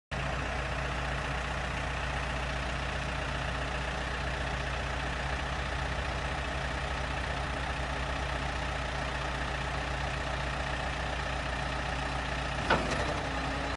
1999 VOLVO L50C For Sale
1999 Volvo L50C Wheel Loader, GENUINE 6785 hours, mostly Forklift Use, 8Tonne Operating Weight, 71.5 Kw Volvo 4 Cylinder Turbo Diesel, Air Conditioned ROPS Cab (air blows cold), Volvo Quick Coupler, Pallet Forks, NEW battery’s, almost unused 1.5m Side Dump Bucket, Hydrostatic Transmission, Third Valve, superb mechanical condition

Just had a major service!

Price NEG